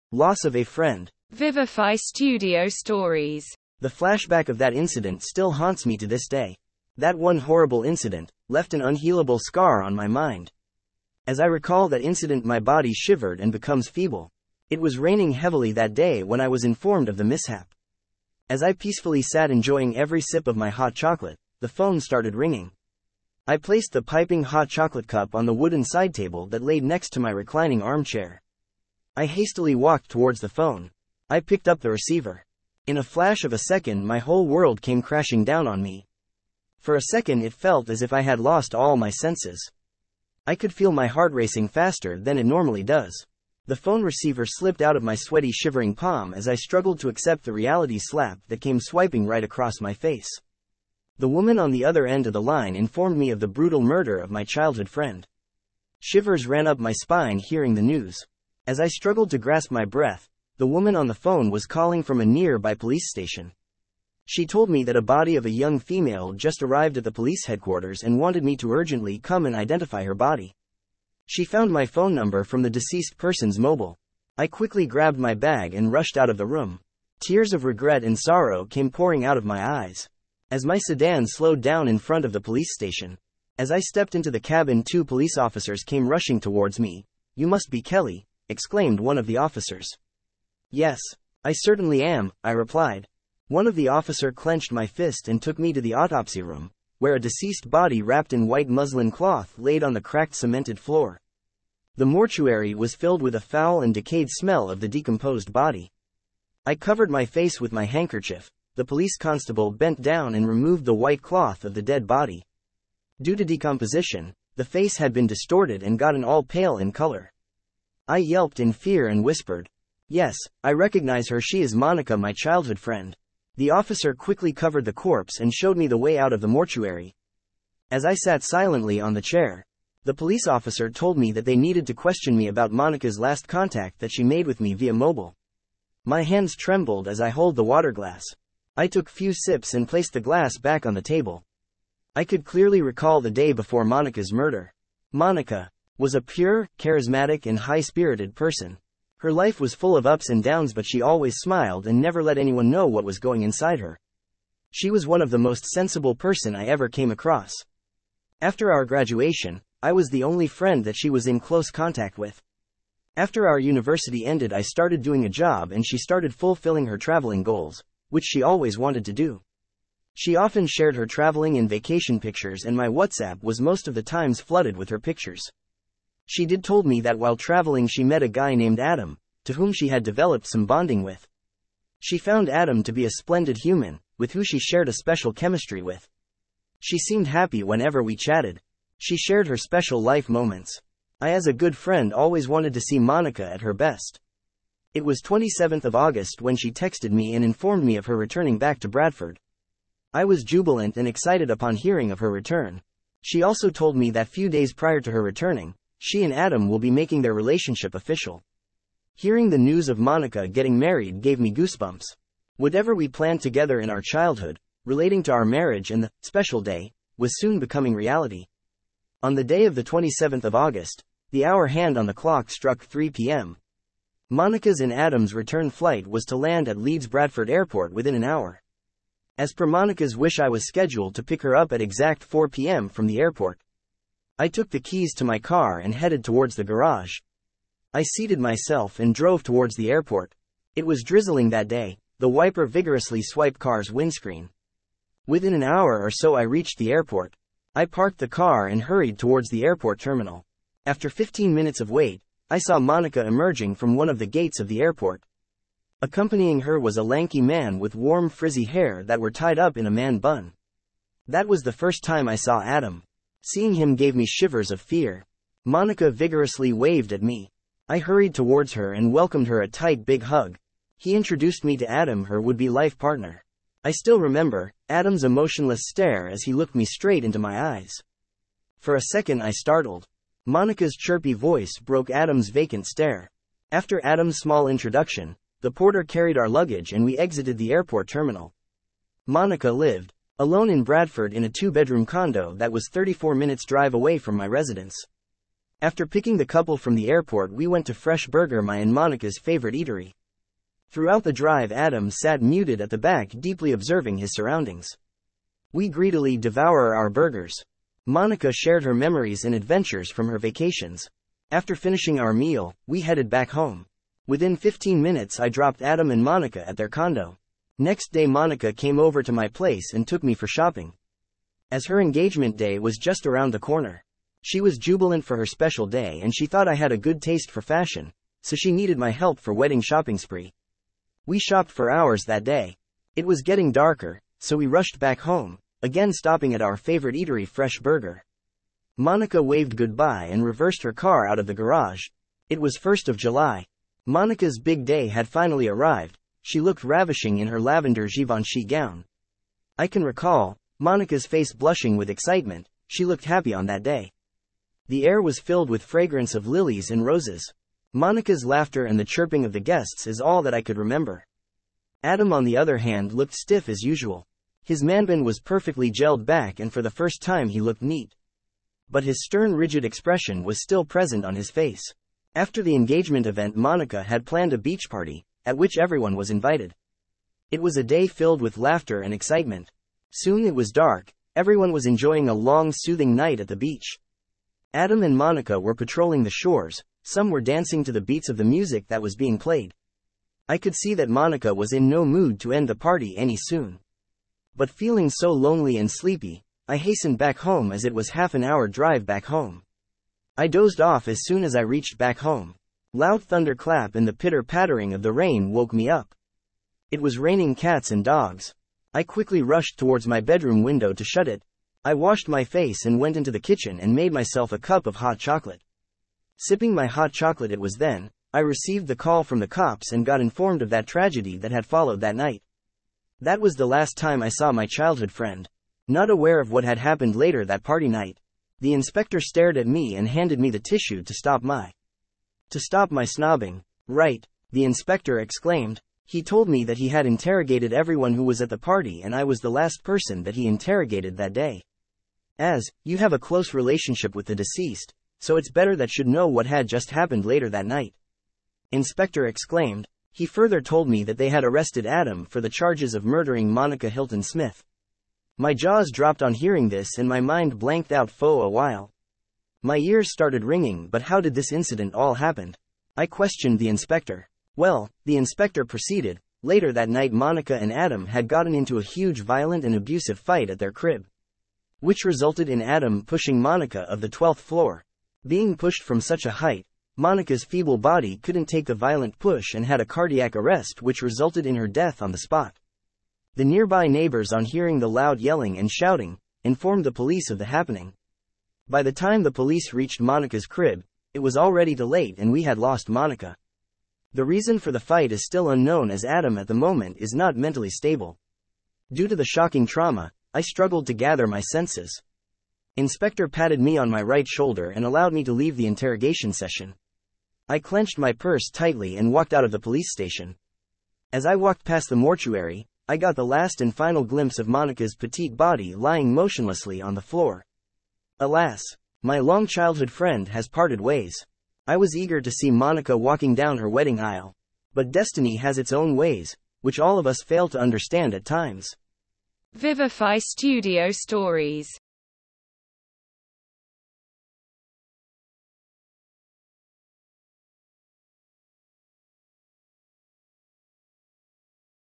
Loss of a friend - Vivify Studio Stories
Loss of a friend, is an adventurous and thrilling story of a murder of a woman. How, the murder changes life of the best friend of the murdered woman and her road towards recovery.